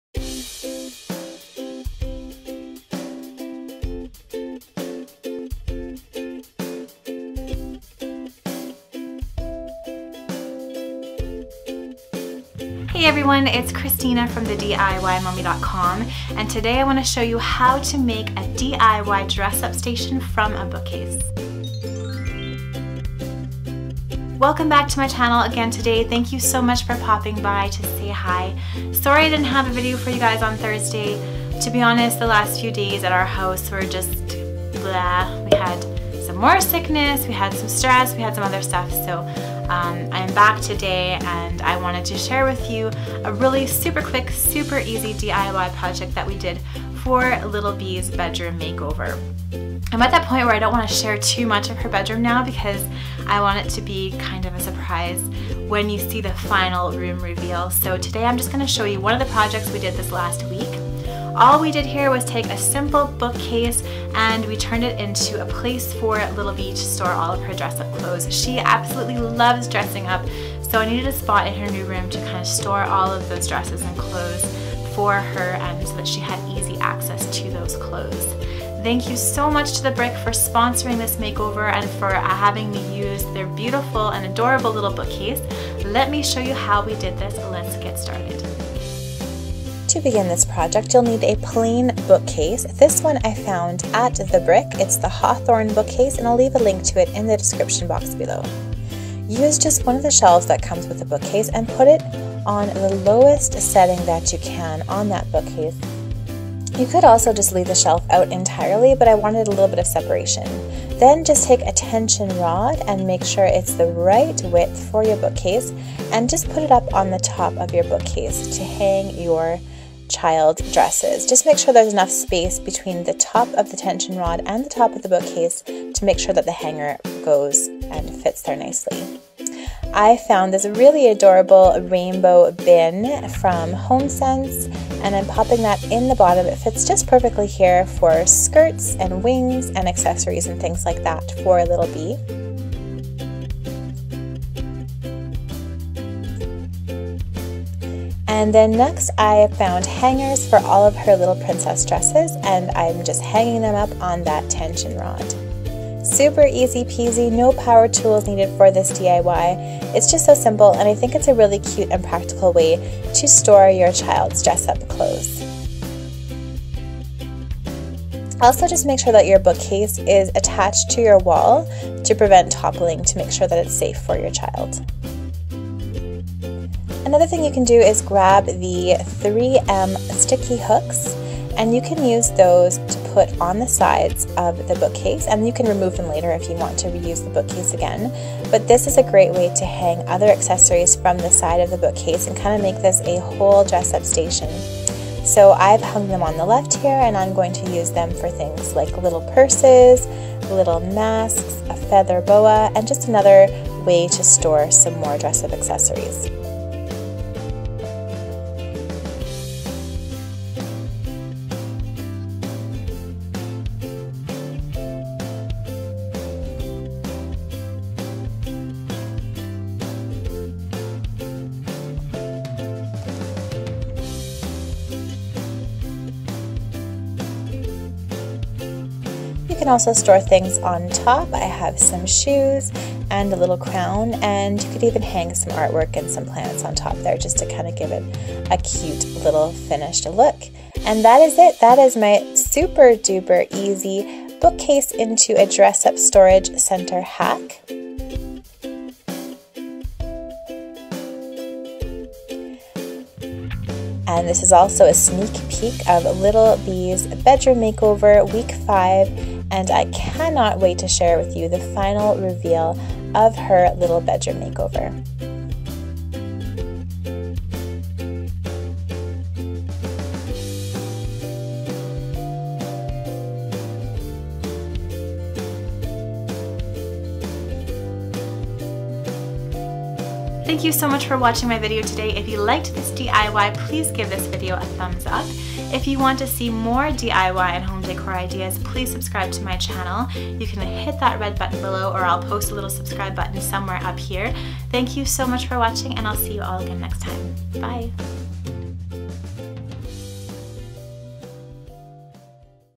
[DIY Channel] Dress-Up Storage Bookcase Hack: Little B's Bedroom Makeover Week 5
Let me show you how to take a bookcase and turn it into a cute little dress-up station for your child's room. This is the easiest DIY ever, and I love how it turned out!

Christina Dennis
Stony Plain AB T7Z 1Y2
CANADA